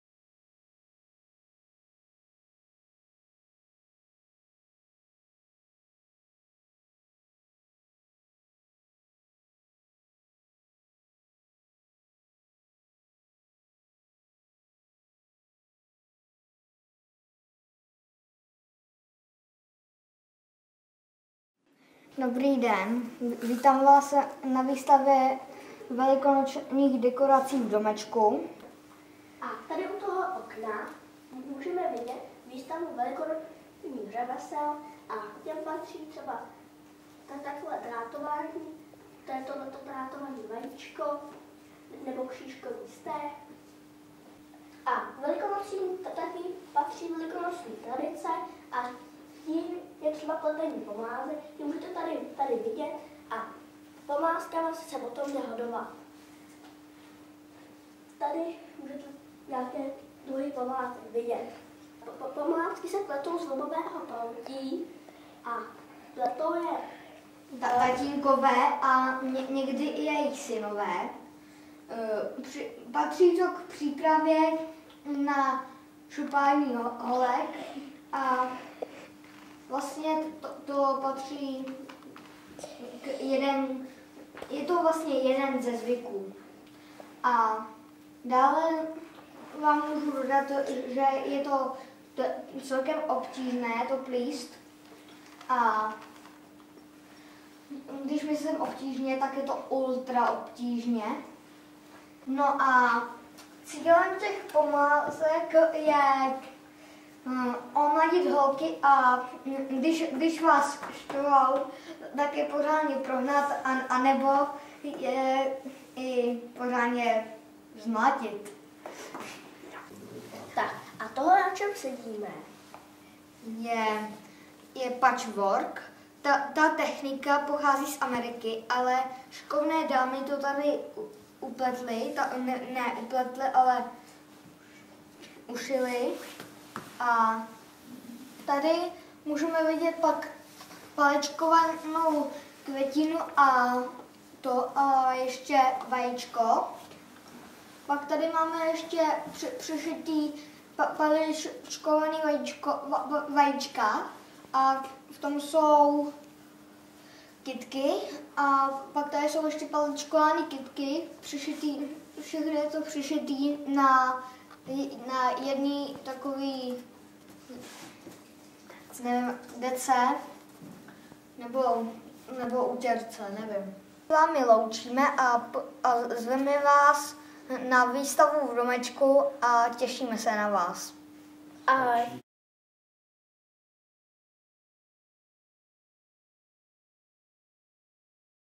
Velikonoční výstava v Domečku
V Domečku právě probíhá velikonoční výstava, která stojí za shlédnutí. Chlapci z Digividea vás za celý Domeček chtějí pozvat. Za pozornost stojí hlavně zasvěcené komentáře moderátorů k výrobě a využití pomlázky.